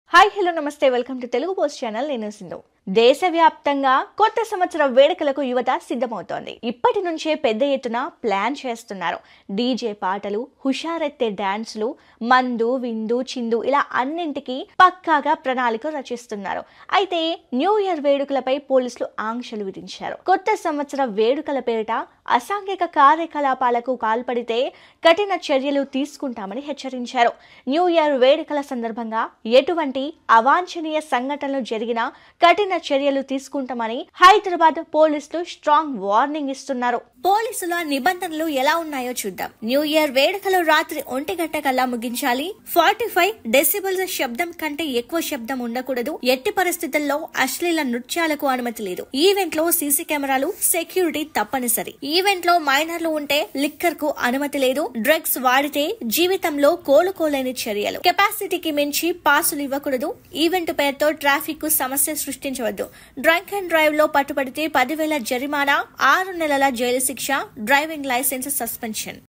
NEW YEAR వేడుకల వేళ ఈ తప్పులు చేస్తే 6 నెలలు చిప్పకూడే? New Year Celebrations Hyderabad | TeluguPost
New Year Celebrations Hyderabad